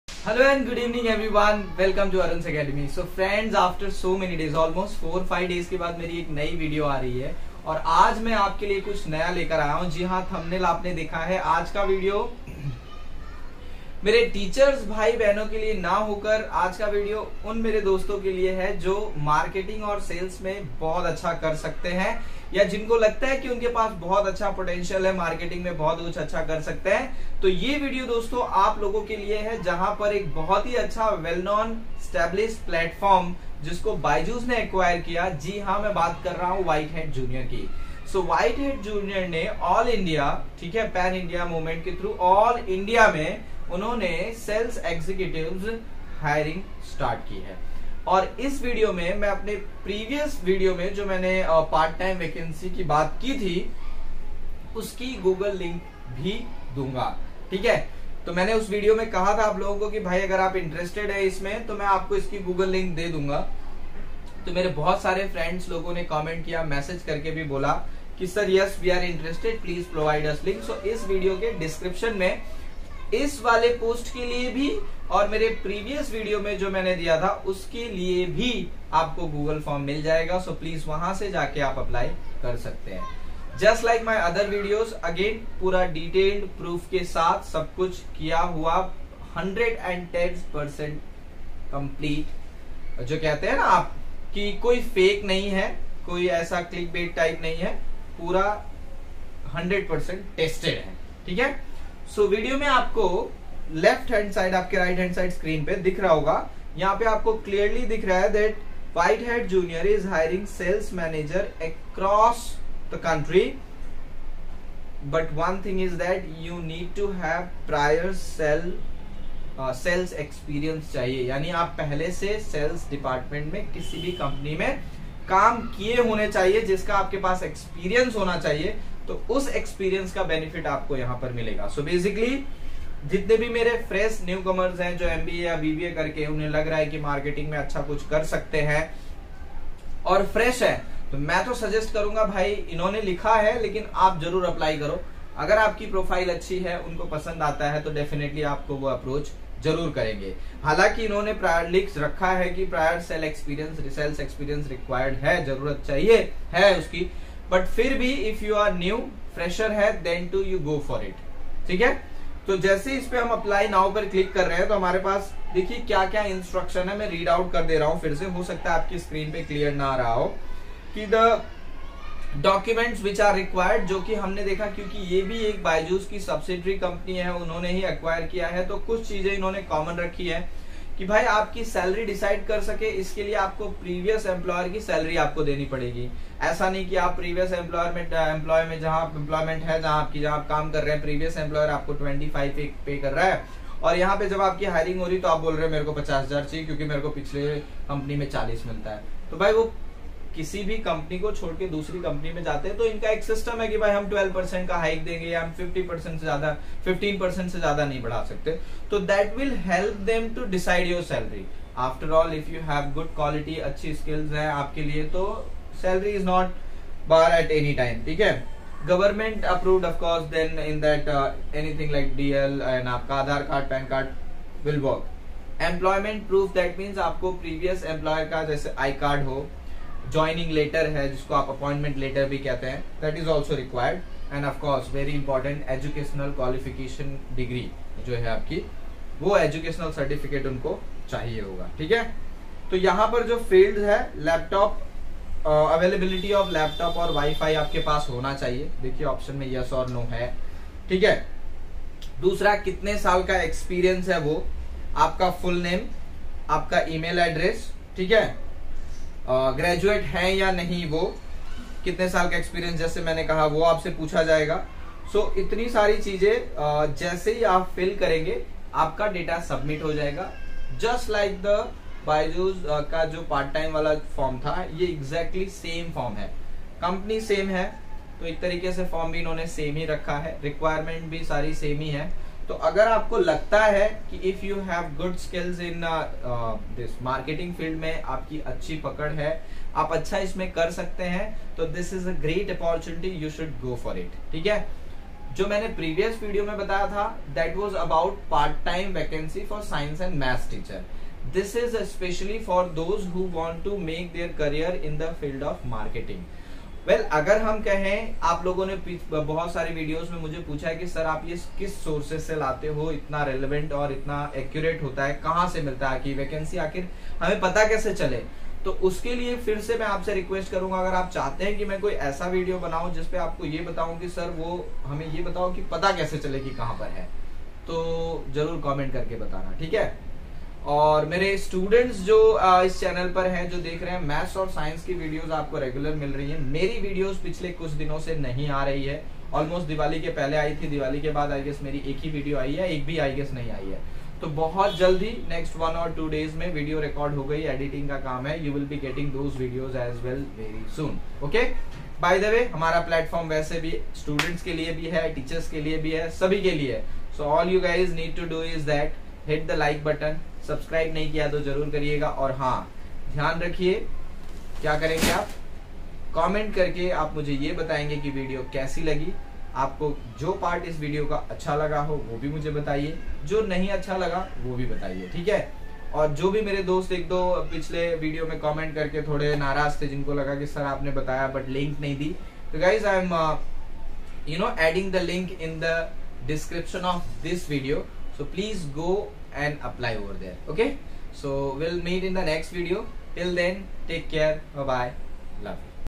New Jobs vacancies in Whitehat Jr. 2021 | Online jobs in BYJU'S
New Jobs vacancies in Whitehat Jr. 2021 | Online jobs in BYJU'S ‎@BYJU'S

Hello  everyone my name is Arun Tiwari and today in this video I have explained the new vacancies available in Whitehat. Jr.
The part time teaching job available for maths and science teacher in Whitehat. Jr. is the latest recruitment process going on.

This video will contain
- The  process
- what are the educational qualification
- freshers for experience

Instagram - English by Arun sir